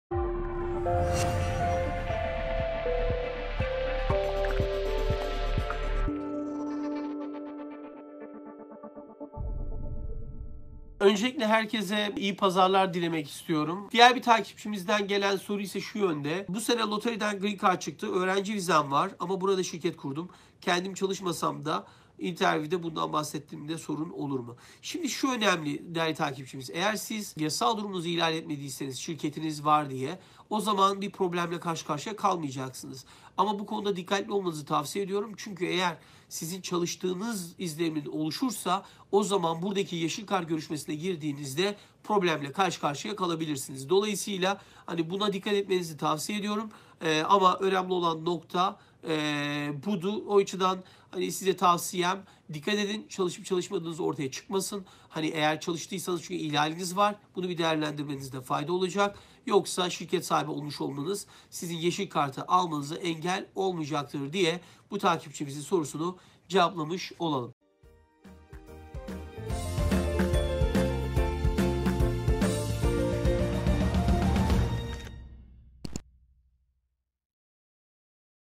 GREEN CARD MÜLAKATINDA DİKKAT EDİLMESİ GEREKENLER NELERDİR ?
Soru: Bu sene Lottery'den Green Card çıktı. Öğrenci vizem var. Ama burada şirket de kurdum. Kendim çalışmasam da var. Interview'de bundan bahsettiğimde sorun olur mu ?